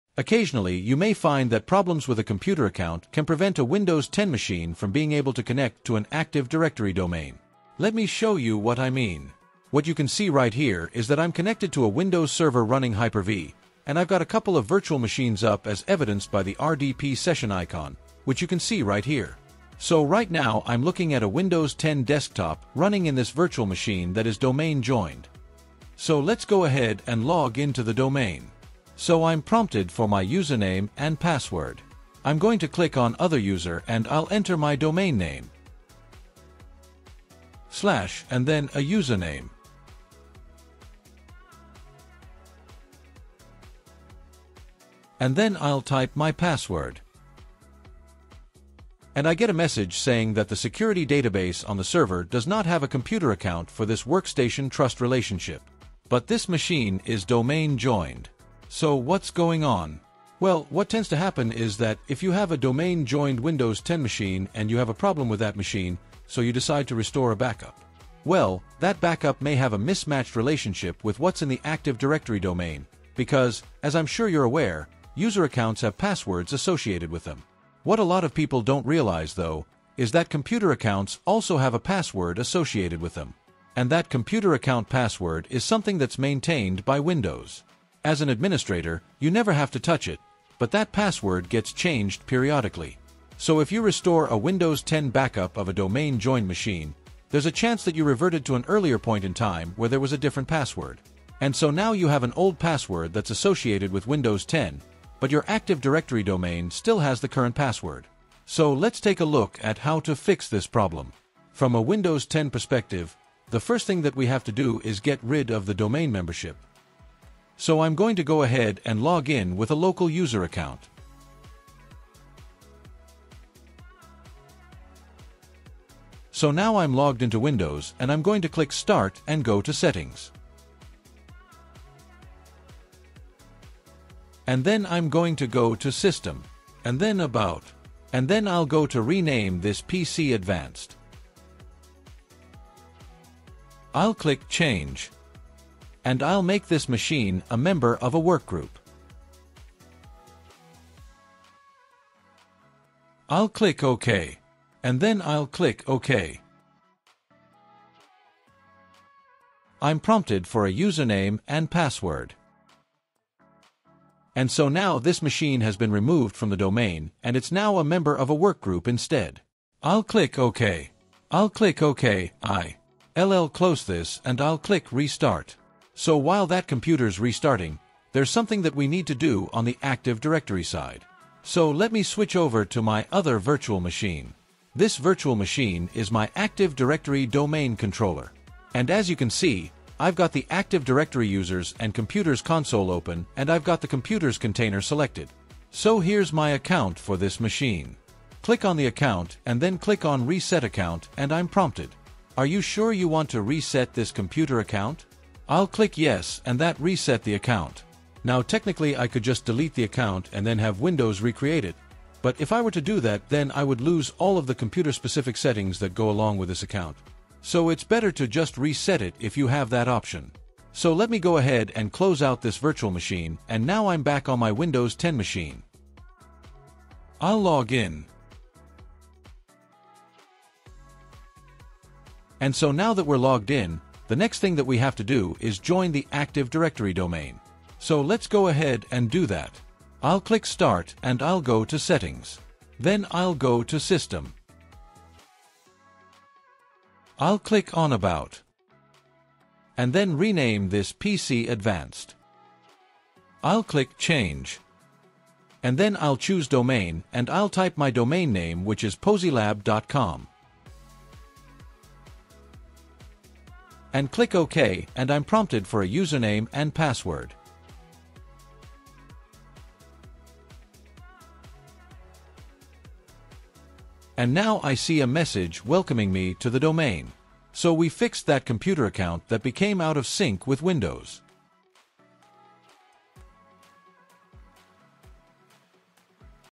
How To Fix Active Directory Domain Login Problem | Windows 10 Advanced Troubleshooting
In this video, we’ll walk you through how to fix Windows 10 AD domain login problems. If you're having trouble connecting your Windows 10 machine to an Active Directory domain, this guide provides simple and effective steps to solve the issue, ensuring smooth domain access.

Perfect for IT support professionals and beginners alike, this troubleshooting tutorial breaks down the process in easy-to-follow language. Watch now to get your system connected and resolve any login issues with AD domains.